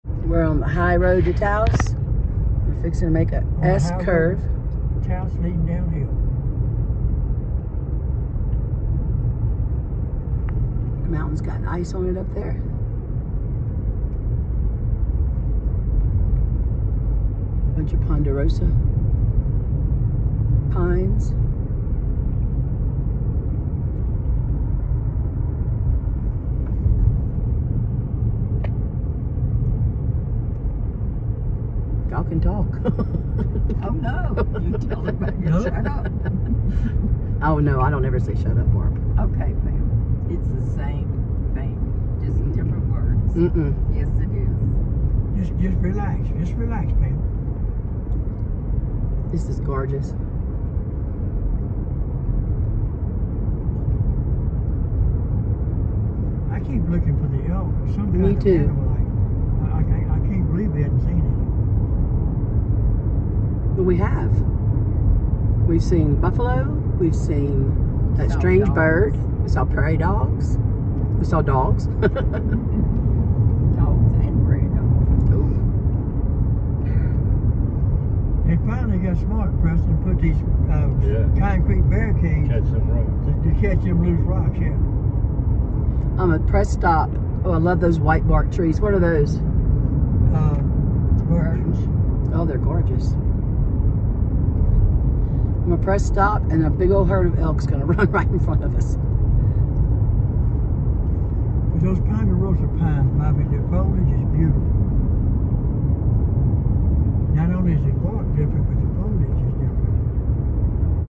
High Road to Taos
Join us on a portion of our ride on the High Road to Taos in the beautiful state of New Mexico. @PawpawSammyAlyBug